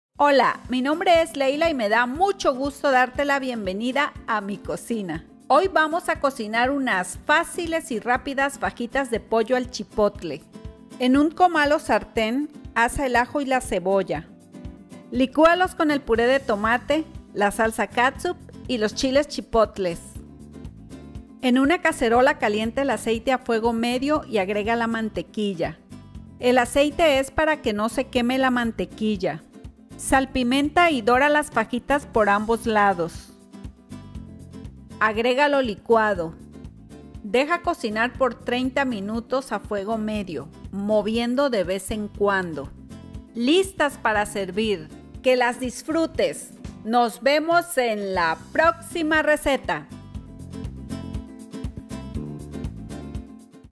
Receta de Pollo al Chipotle. CANTIDADES EN LA CAJITA DE INFORMACIÓN
Receta de Pollo con Chile chipotle. Cocina Mexicana

INGREDIENTES

1 kilo de pechuga de  pollo en fajitas

1 taza de puré  de tomate condimentado

1 taza de salsa catsup

1 lata pequeña  de Chiles  chipotle adobados (  son 105 gramos,  le puedes poner más o menos dependiendo del punto  picoso que que te guste )

2 dientes de ajo asados en un comal o sartén

1/4 de cebolla asada en un comal o sartén

1 cucharada sopera  de aceite  ( para que no se queme la mantequilla)

2 cucharadas soperas  de mantequilla ( 30 gramos)